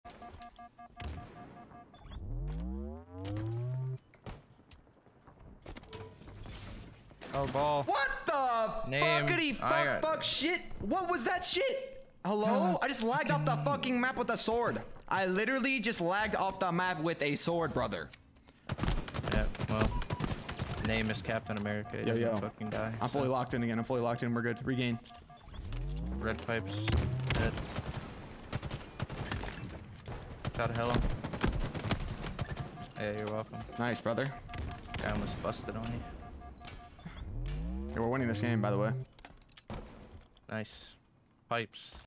OpTic Trippey Gets Checked Out By 343 But Quickly Regains!!
Trippey gets some crazy lag!